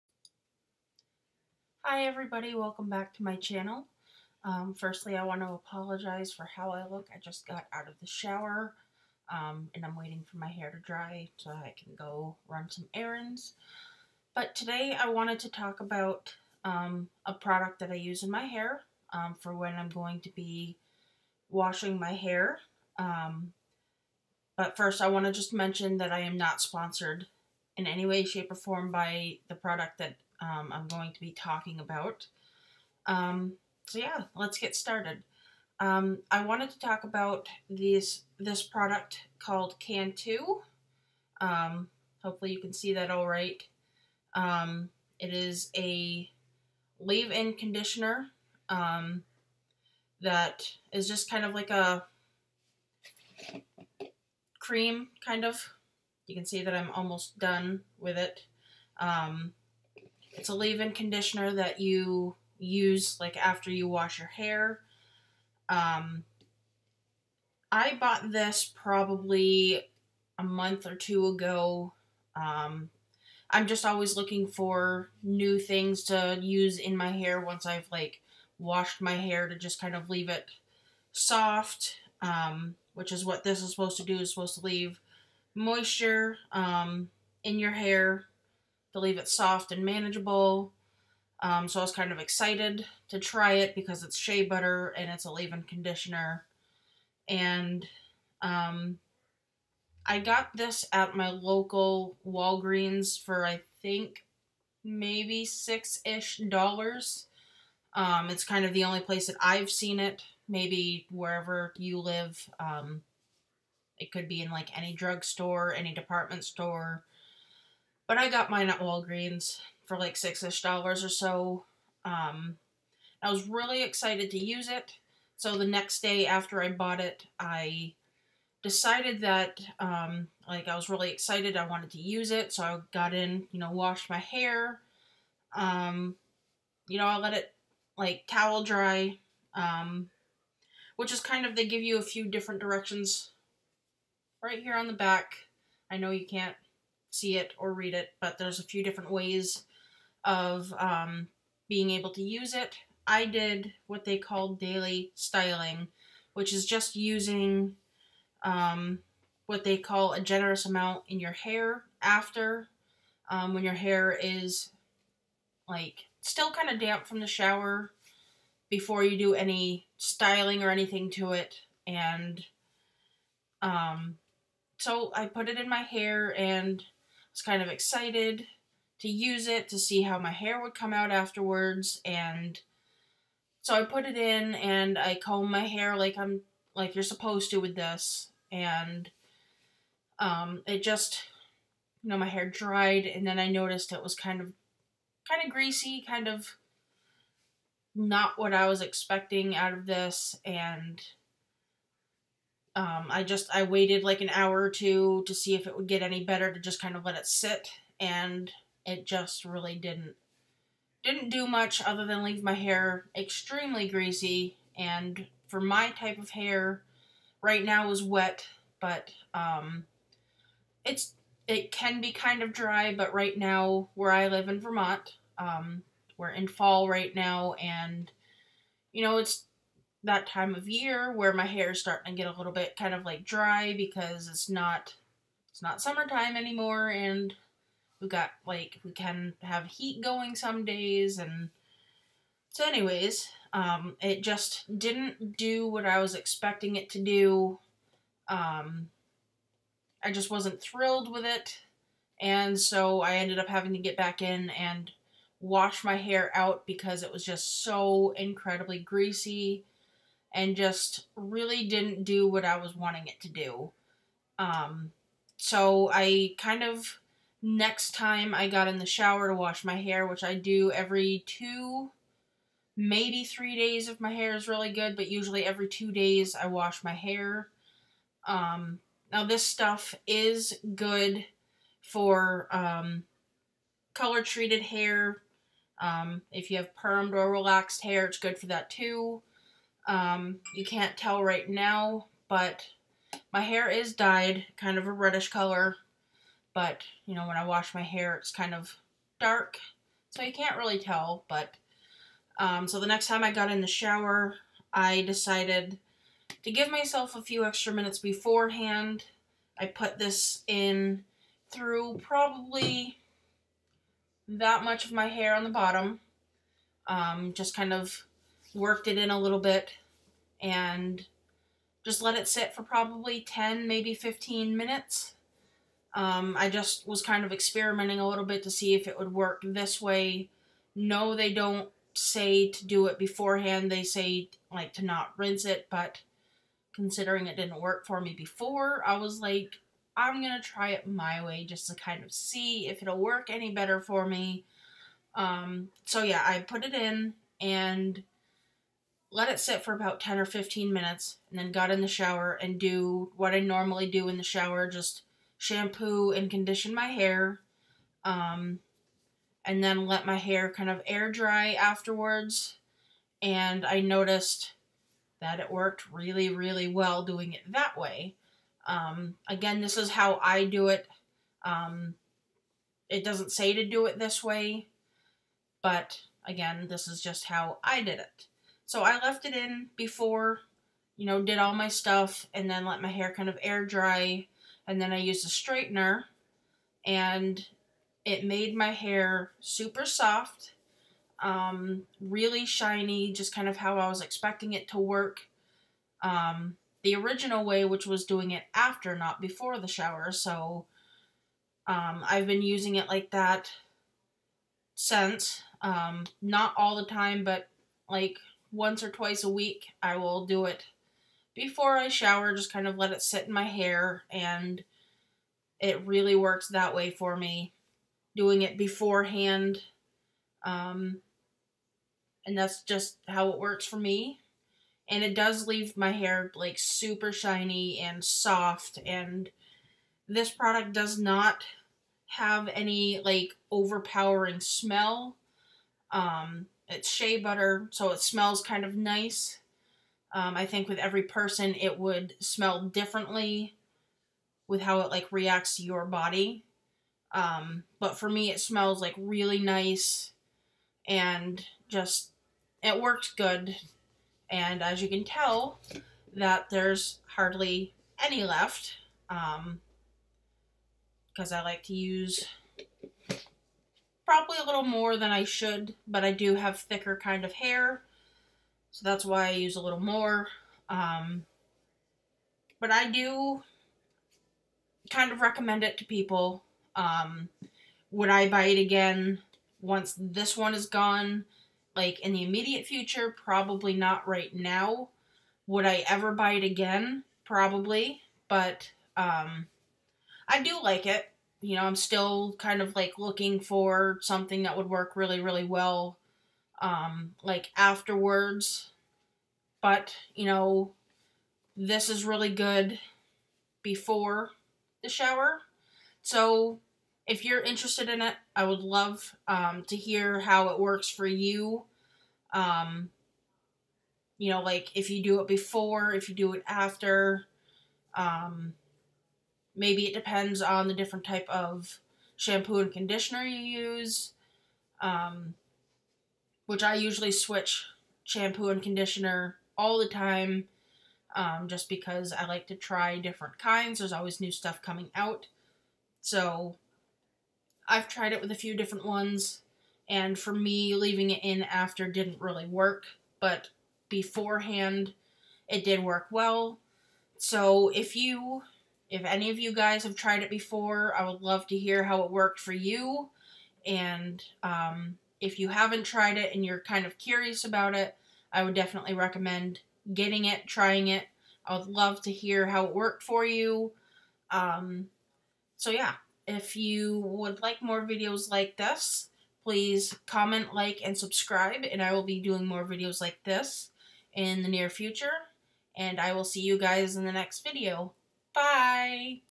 CANTU Leave in Conditioning Review
Cantu Leave In Conditioner Review. What I think about it and how I use it.